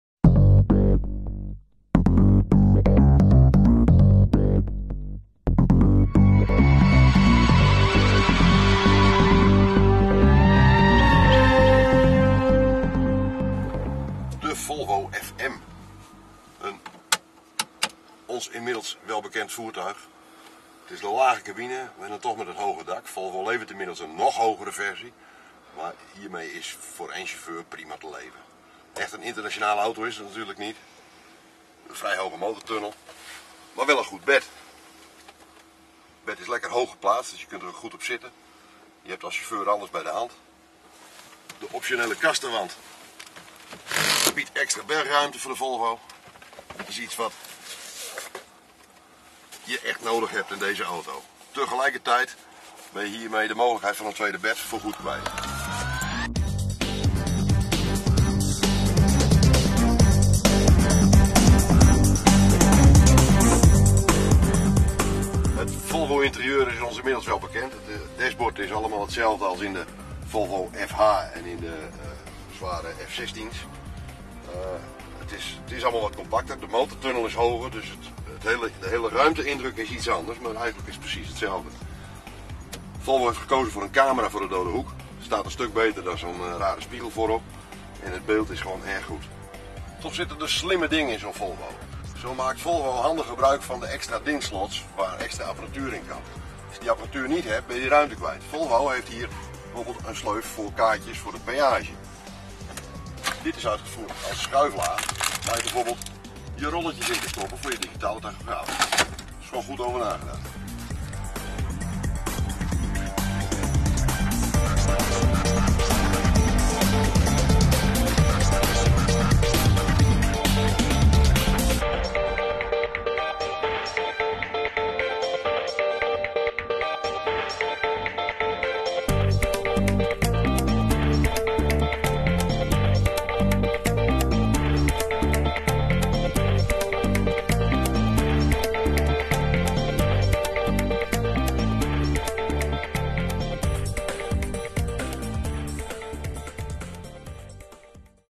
Praktijkest Volvo FM11 410
Truckstar test de Volvo FM 11 met 410 pk. We rijden onze Ardennenroute met onze eigen trailer en veertig ton totaalgewicht.